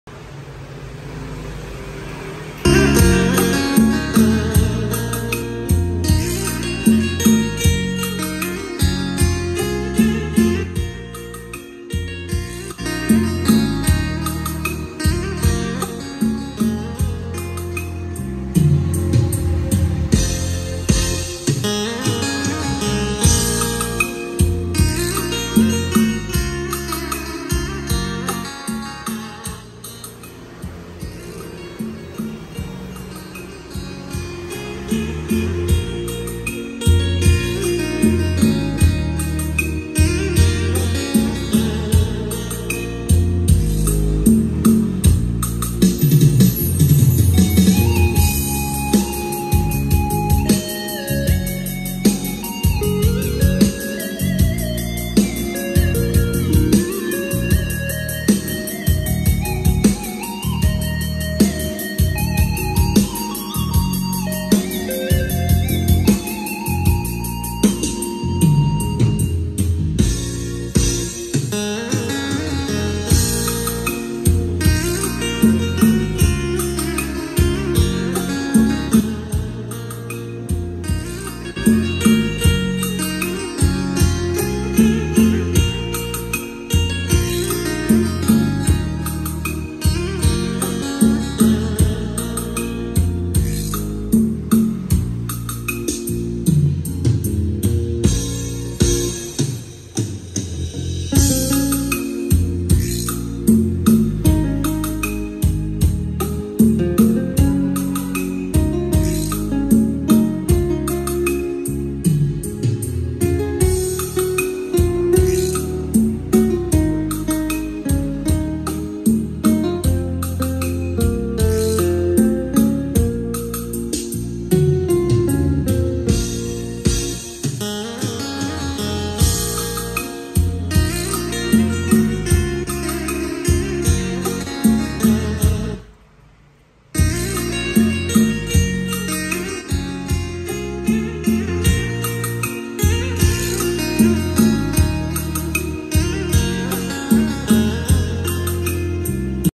Lọc Xì Technics SH-8045 Japan(GIÁ 3tr5)
💪 Mỏng nhưng tiếng không hề Mỏng tínào
✅Lọc Technics  SH-8045
24 Cần chất âm quá hay ,Mạnh và dày tiếng
Dá : 3tr5
Chốt gọi e ship nha